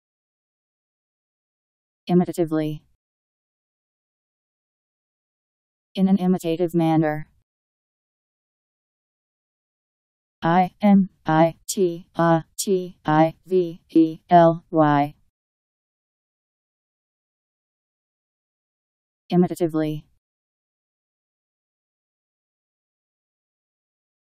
Imitatively Meaning
Video shows what imitatively means. In an imitative manner..  Imitatively Meaning. How to pronounce, definition audio dictionary. How to say imitatively. Powered by MaryTTS, Wiktionary